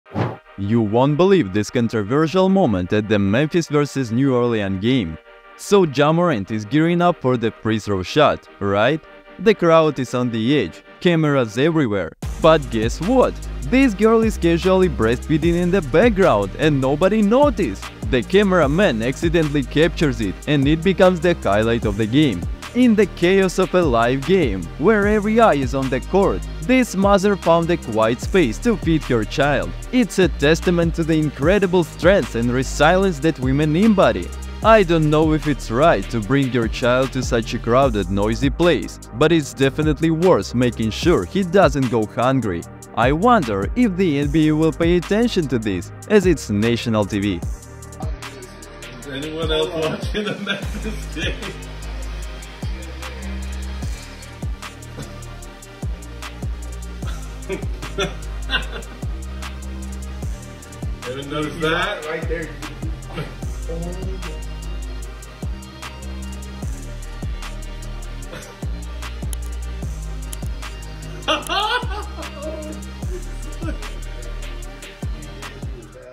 This Girl Breastfed her Baby in Front of Ja Morant!
you won't believe this controversial moment at the Memphis vs. New Orleans game! So, Ja Morant is gearing up for that free throw shot, right? The crowd's on edge, cameras everywhere.
But guess what? This girl's casually breastfeeding in the background, and nobody notices